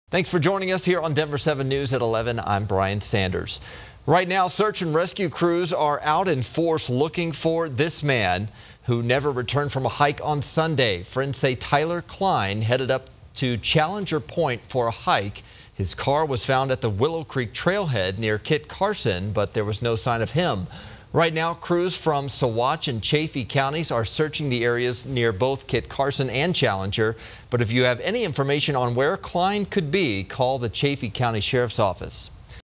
Search continues for man missing since Sunday after likely hiking Kit Carson Peak, Challenger Point
Officials are searching for a missing man who may have been hiking in the area of Kit Carson Peak and Challenger Point in Saguache County over the weekend.